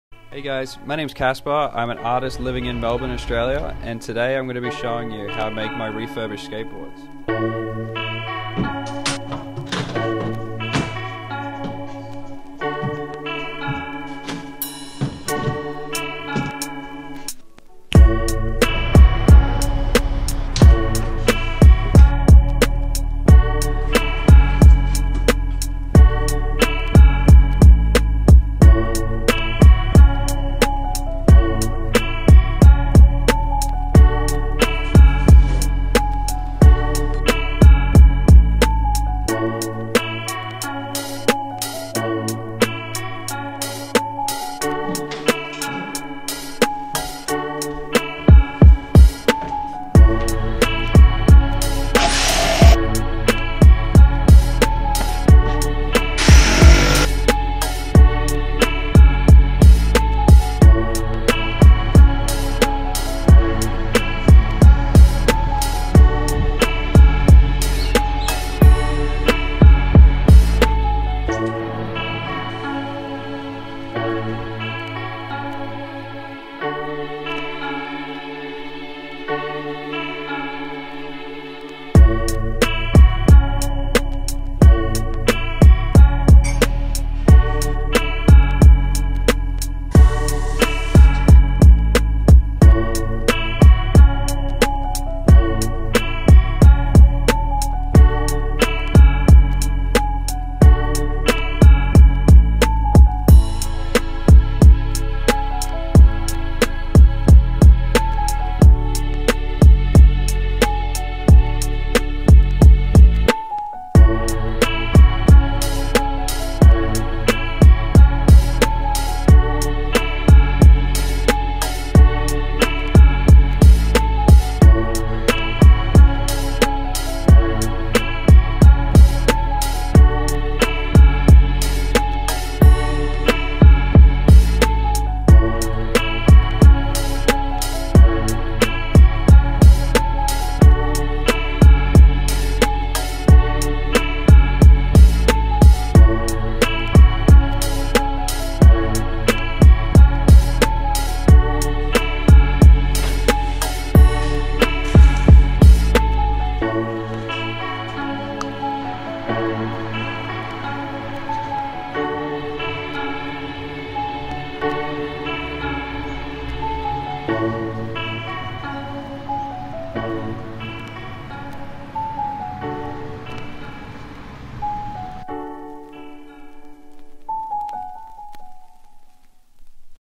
HOW I PAINT & CARVE MY REFURBISHED SKATEBOARD ART - KASPER
Here's a step by step guide on how I create my skateboard decks, these techniques could be great for those use; jigsaws and airbrushes but either way I'm sure you'll find a little trick to keep with you!